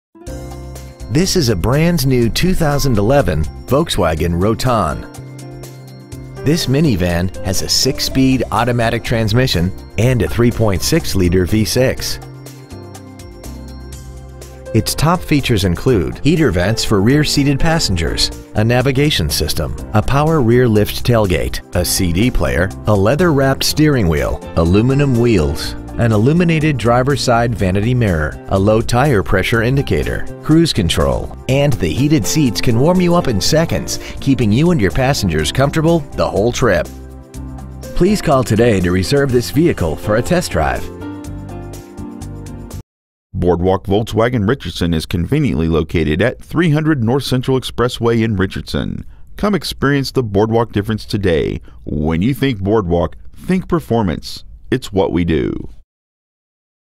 2011 Volkswagen Routan Richardson TX 75080
Year : 2011

 Make : Volkswagen

 Model : Routan

 Engine : Gas V6 3.6L/

 Trans . : Automatic

 Exterior : Calla Lily White

 Miles : 5

 Interior : Sierra Stone

 Stock : 11U006

 300 North Central Expressway

 2011 Volkswagen Routan Richardson TX